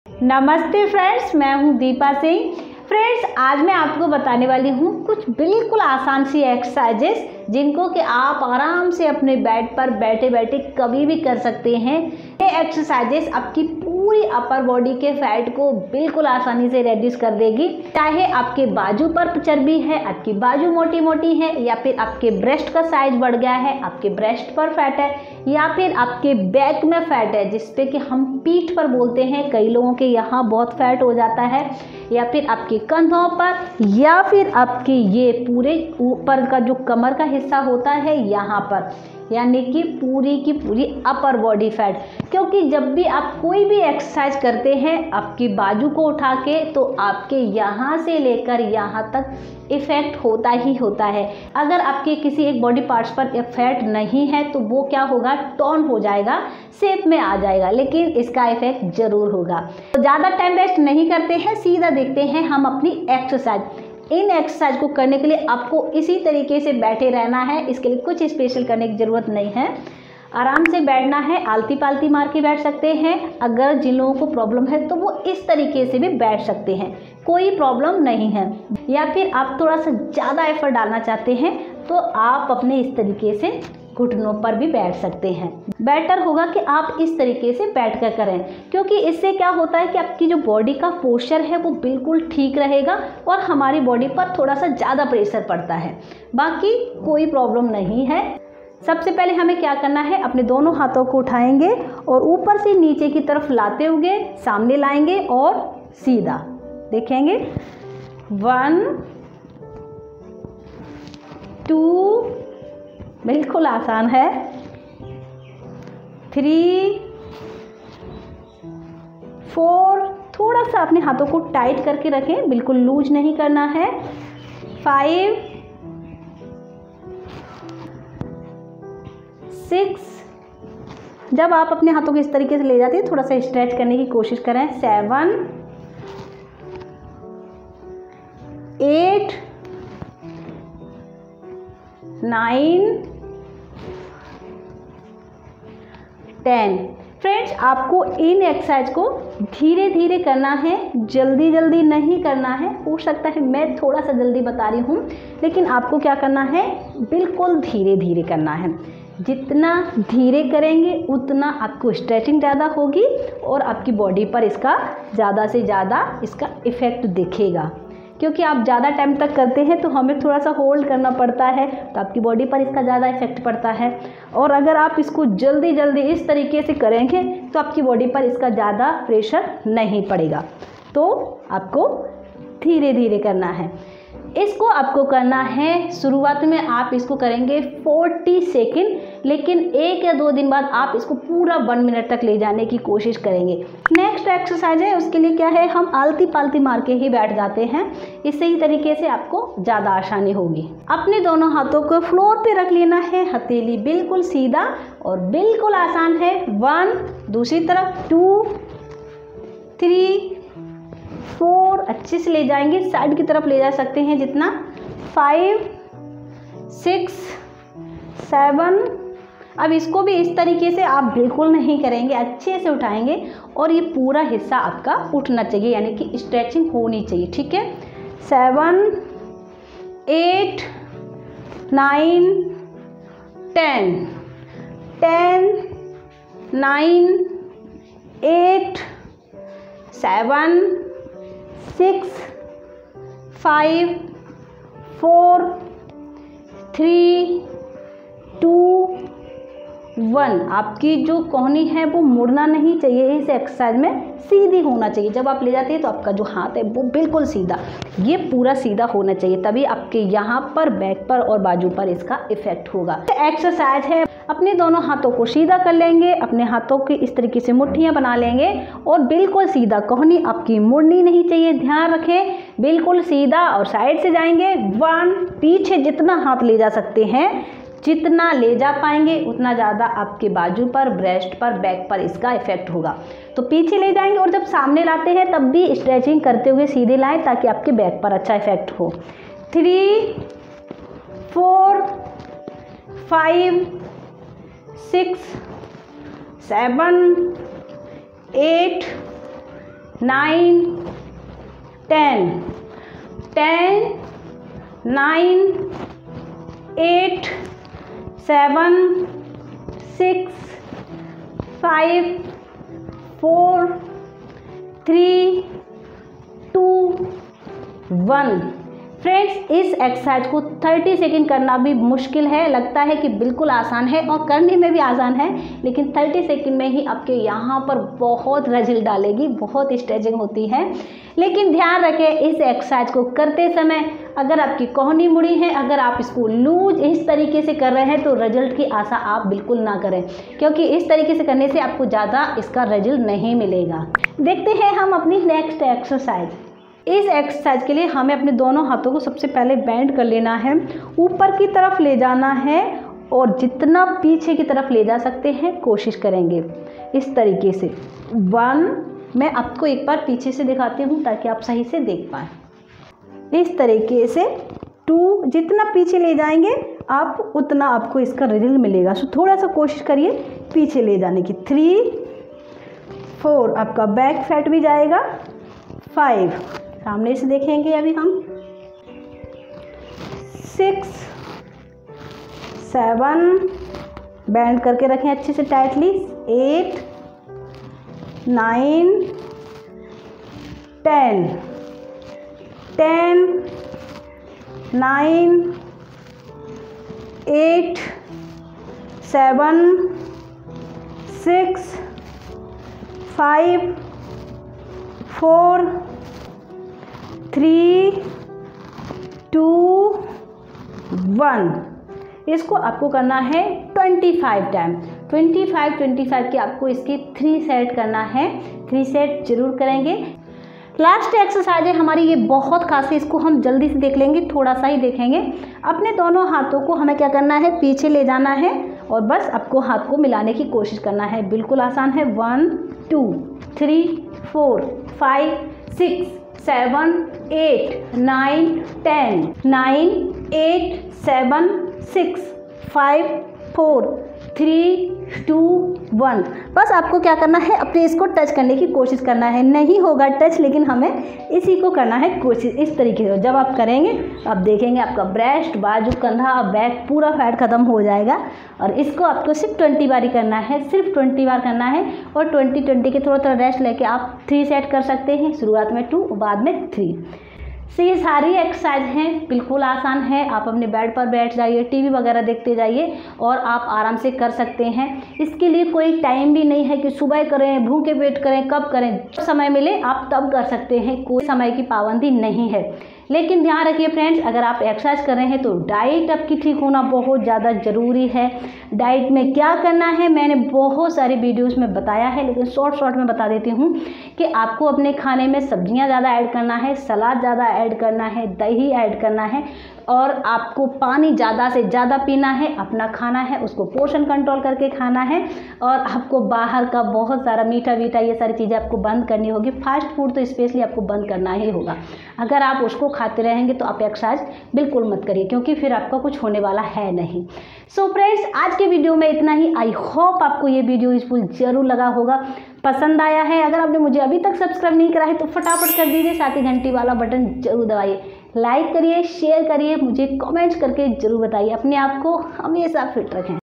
सिर्फ 5मिनट में बाजू की चर्बी,पीठ(Back),ब्रेस्ट चर्बी,कंधे चर्बी निकल जाएगी/UPPER BODY FAT/Easy Exer
सिर्फ 5 मिनट में बाजू की चर्बी , पीठ (Back) की चर्बी ,ब्रेस्ट की चर्बी , कंधे की चर्बी निकल जाएगी
UPPER BODY FAT
Easy Exercise
ARM FAT, BREAST FAT, BACK FAT, SHOULDER FAT